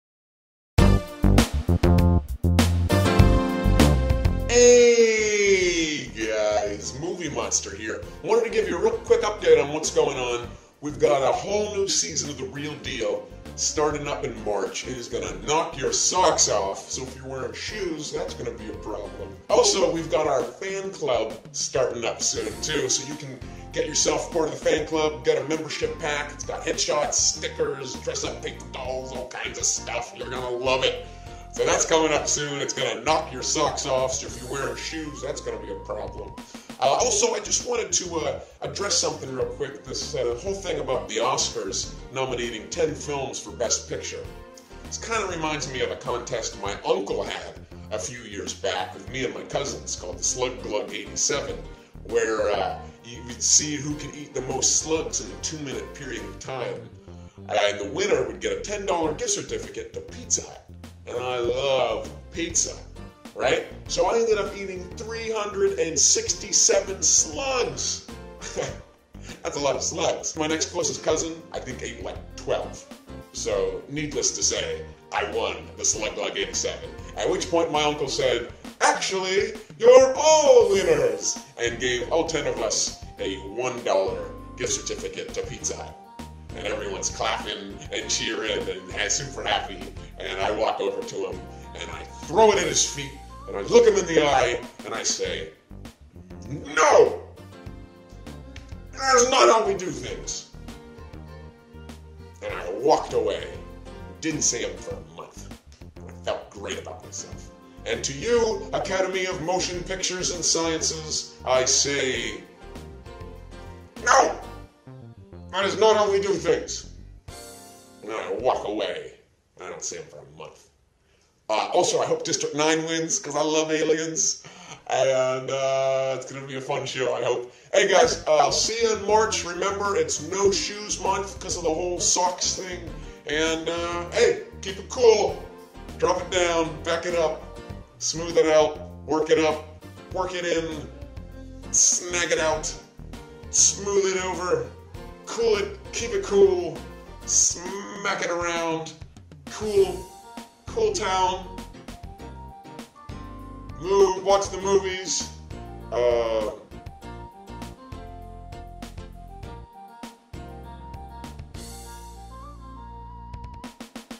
Movie Monster Vlog: Slug Glug '87
Movie Monster gives an update about the new season, as well as some musings on the upcoming Oscars.